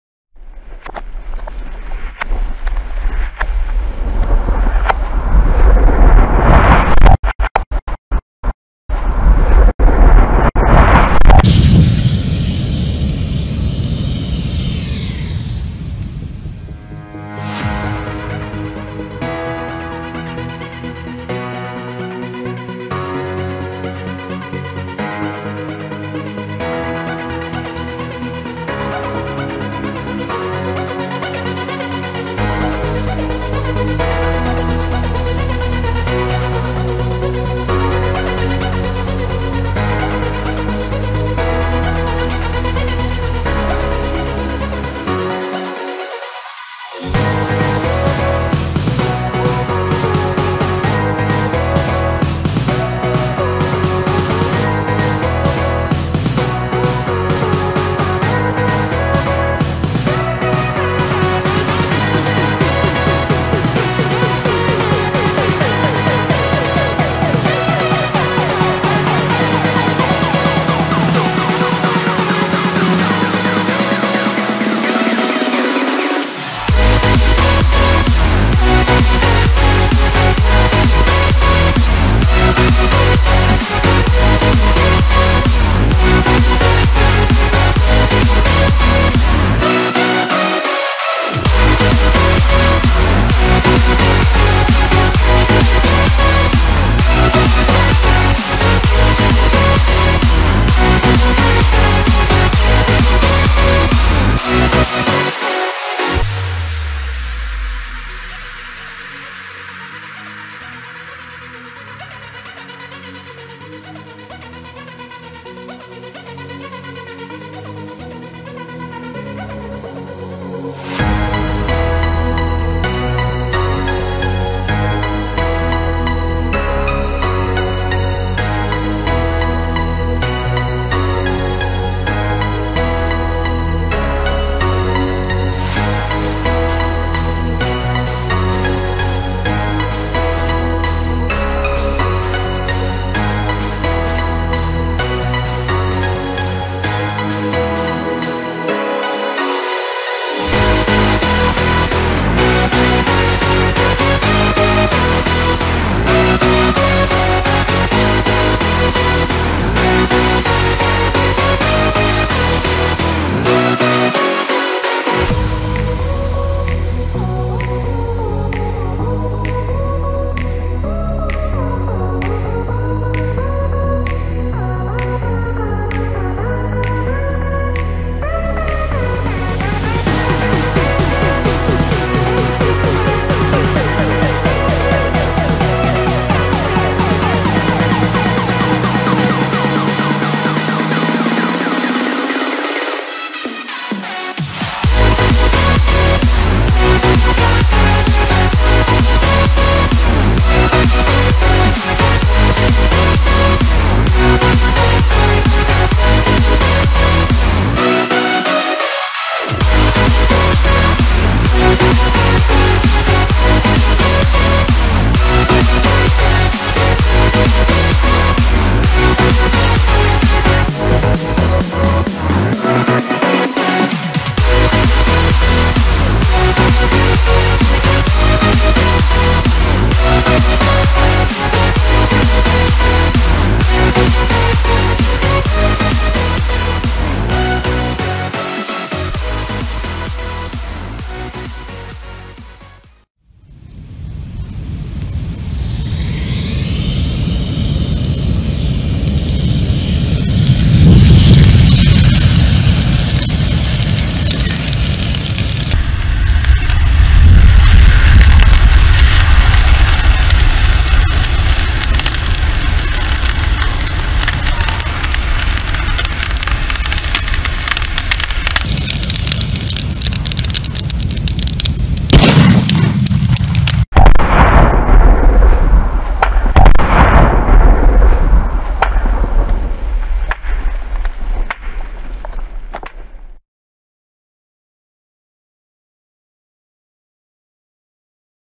The Cabin in the Woods [Fire Chain reaction and EXPLOSION]
This litle house was created from 250 pcs of wood slices.
The main source of the fire was gunpowder inserted into the center of the house, and pyrotechnics also played a role on the end.